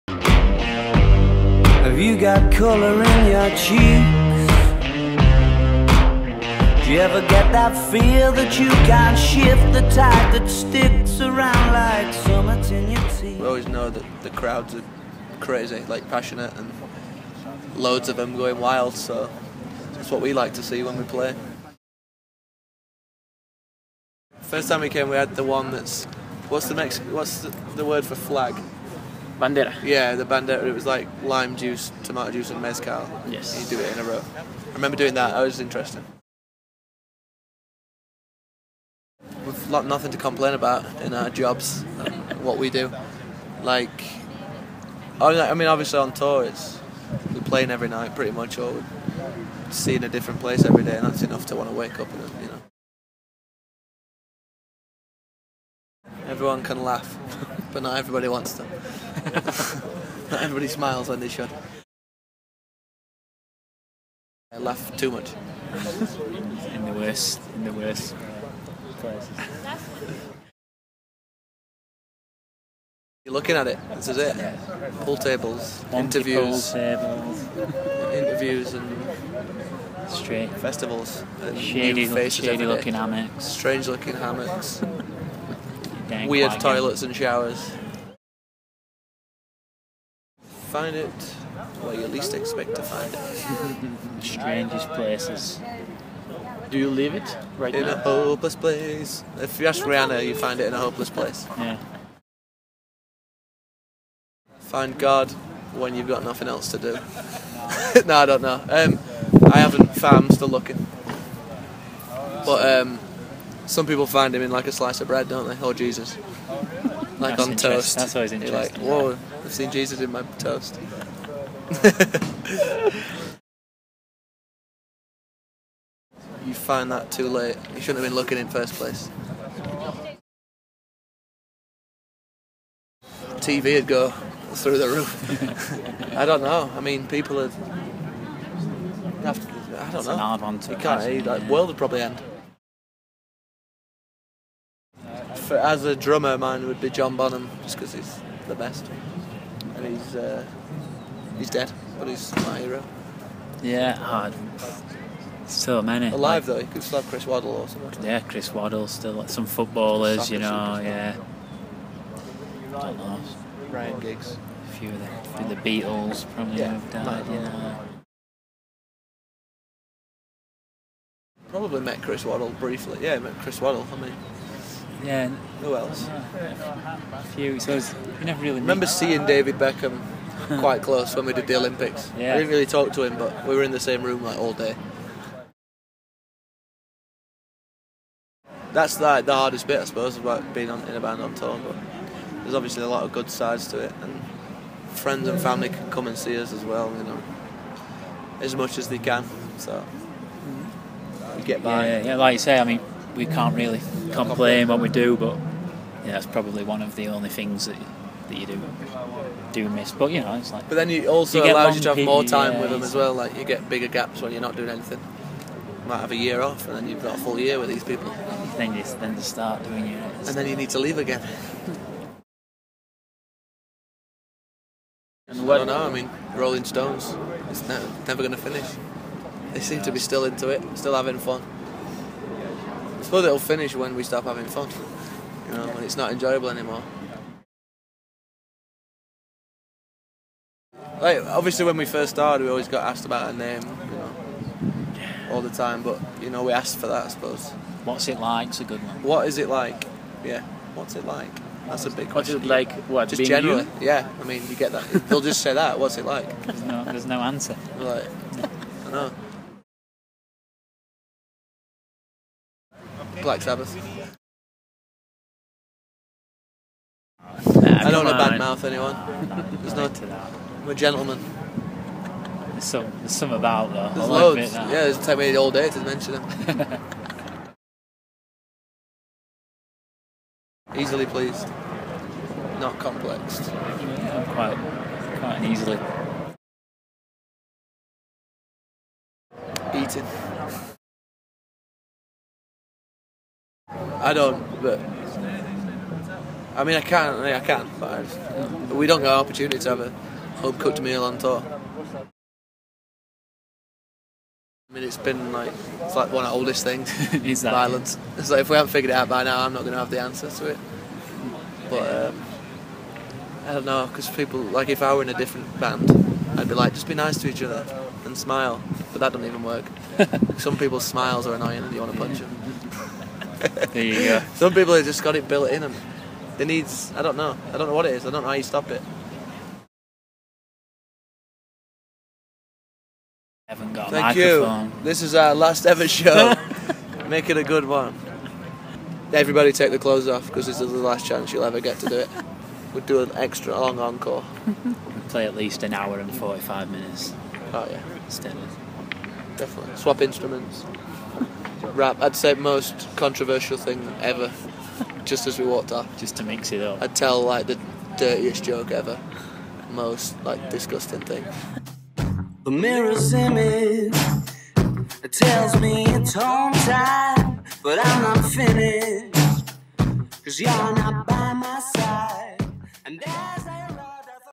Bckstg con Arctic Monkeys
Una de las bandas más esperadas del CC13 nos recibió afuera de su camerino. Aunque son hombres de pocas palabras, Matt y Jamie compartieron algunas cosas con nosotros, ¿quieres saber de qué hablamos?